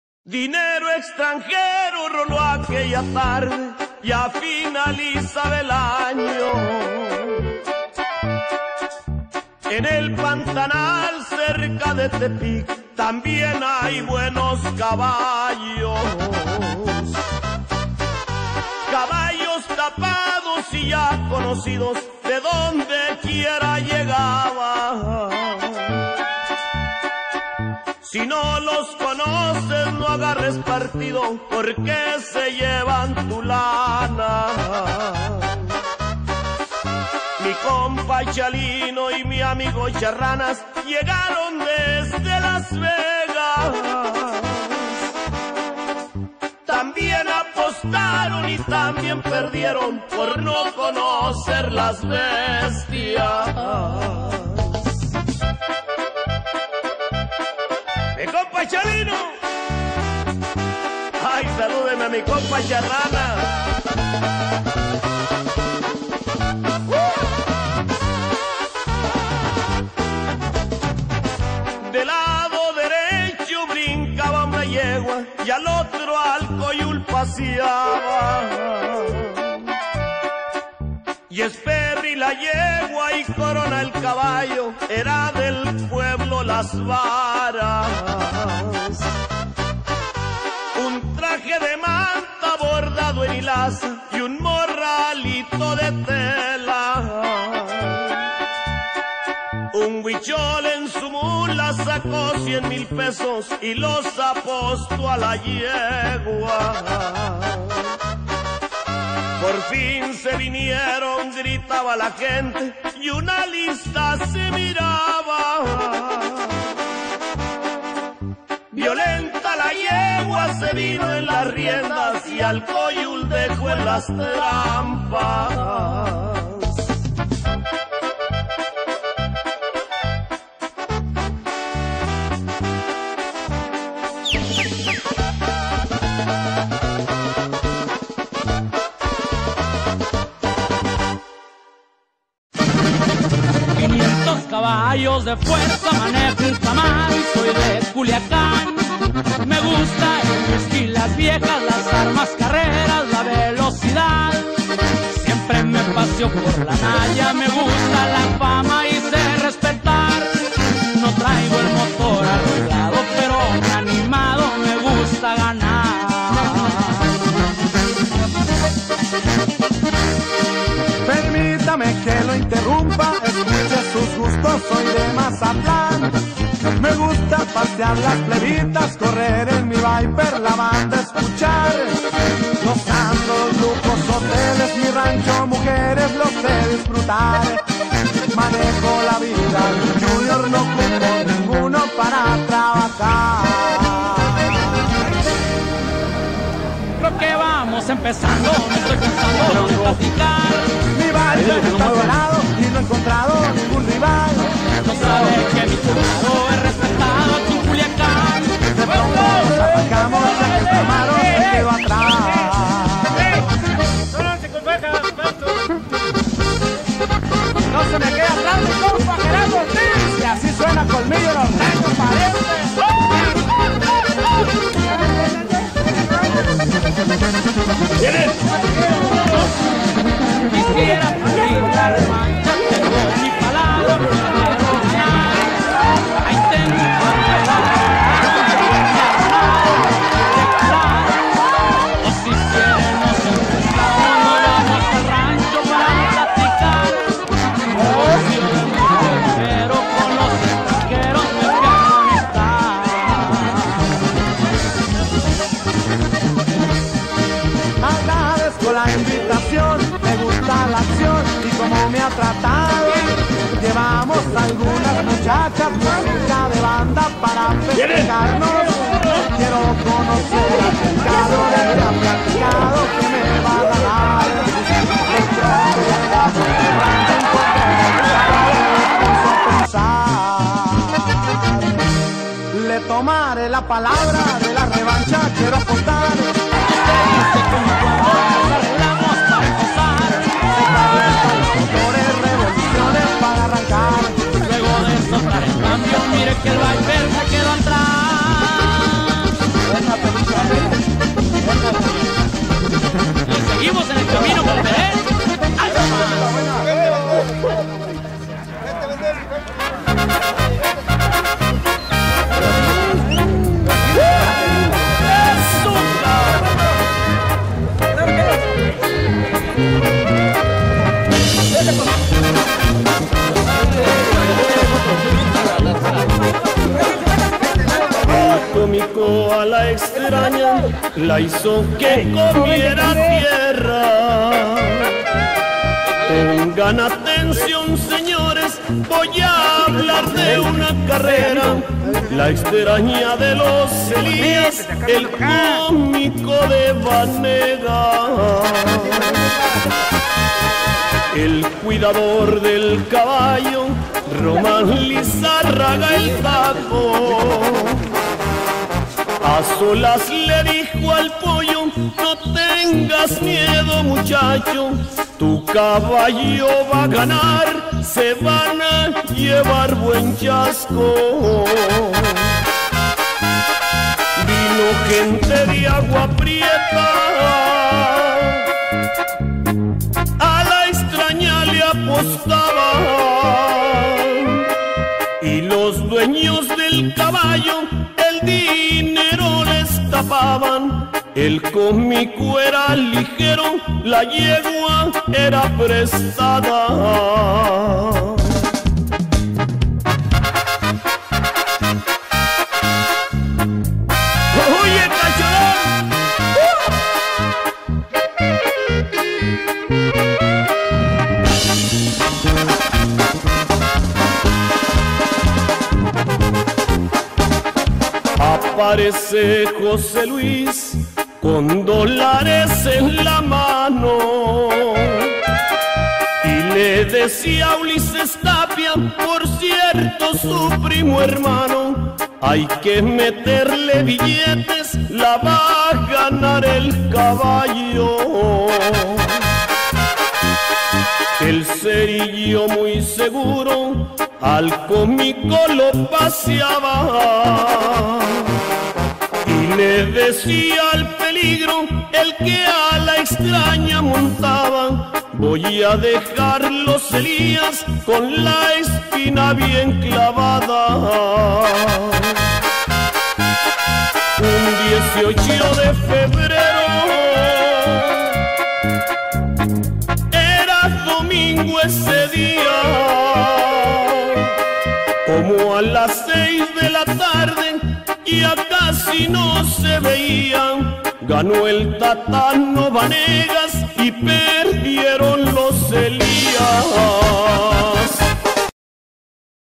LA JAROCHA vs EL PICHON
CARRERA REALIZADA EN LOS CARRILES DE SAN LAZARO ETLA OAXACA.
CARRERON SEÑORES A 250 VRS. TODO PAREJO FAVORITO EL PICHON DE PUEBLA CON FAJA AGARRABAN PERO NUNCA PENSO QUE ESTA YEGUA LE GANARA Y MUCHO MENOS LO BORRARA DE LA FOTO MUY BUENA CARRERA.